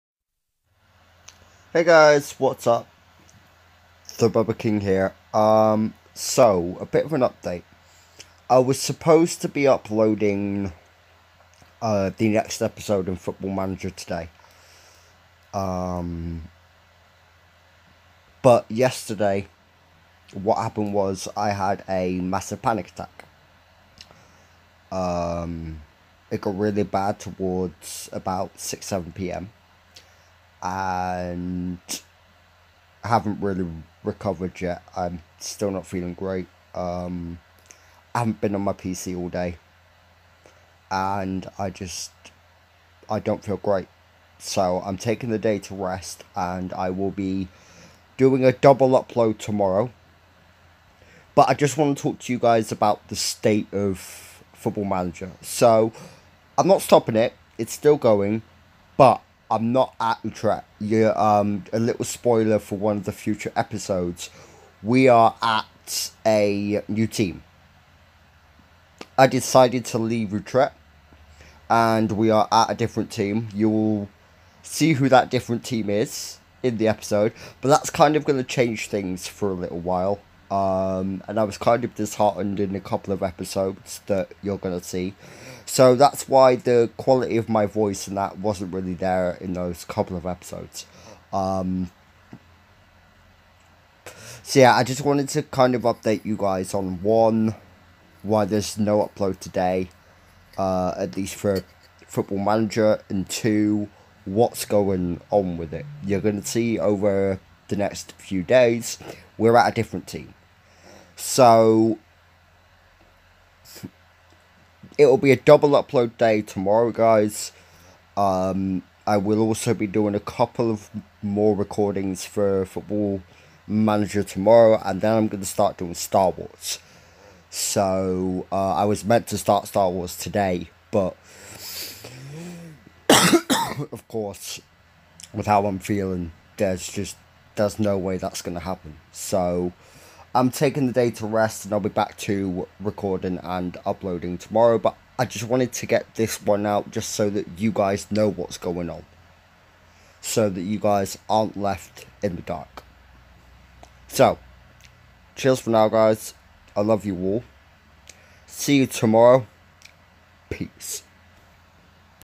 An Update and the state of football manager 2017
TheBubbaKing gives an update on how hes feeling, the reason for missing an upload day and the state of the Football Manager series